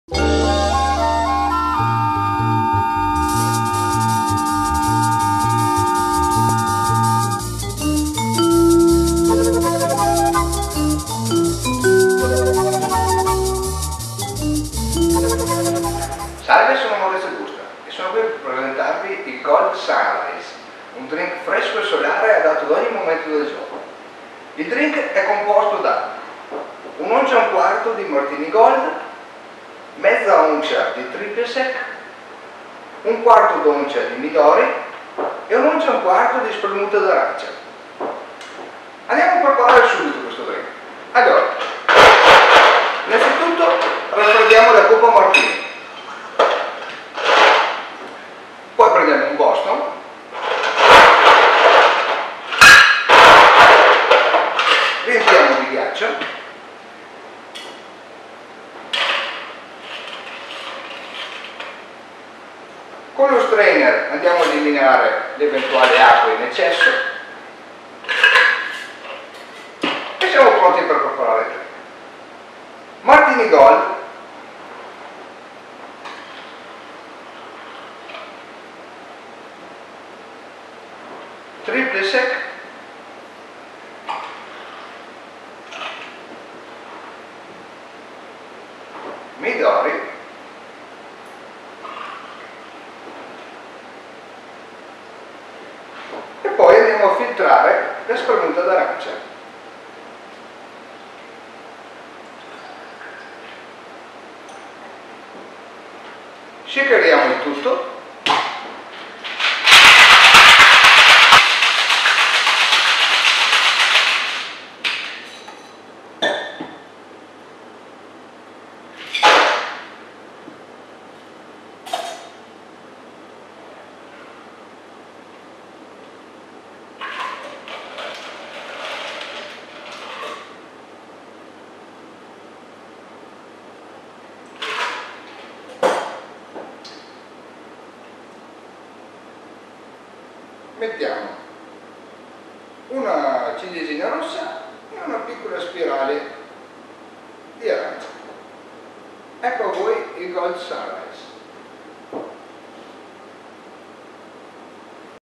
Maurizio Busta prepara il Gold Sunshine
Maurizio Busta vi mostra come preparare un ottimo drink basato su Martini Gold. Una bevanda semplice, dal gusto fruttato ma ricca di aromi e fragranze.